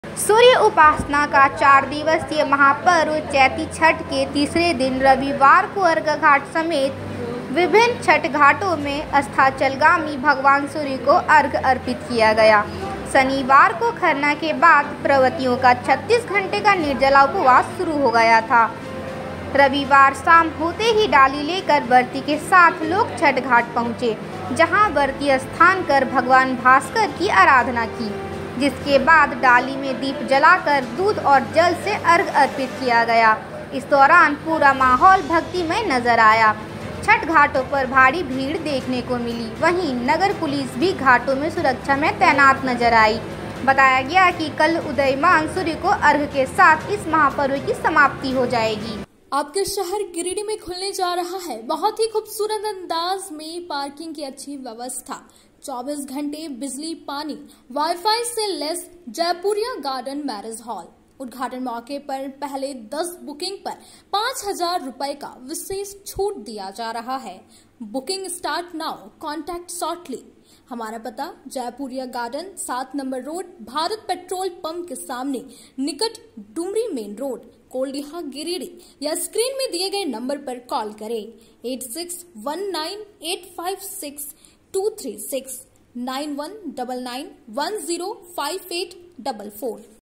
चैती छठ के तीसरे दिन अरघाघाट समेत विभिन्न छठ घाटों में भगवान सूर्य को अर्घ्य अर्पित किया गया।
सूर्य उपासना का चार दिवसीय महापर्व चैती छठ के तीसरे दिन रविवार को अरघाघाट समेत विभिन्न छठ घाटों में अस्थाचलगामी भगवान सूर्य को अर्घ्य अर्पित किया गया।Newslinegiridih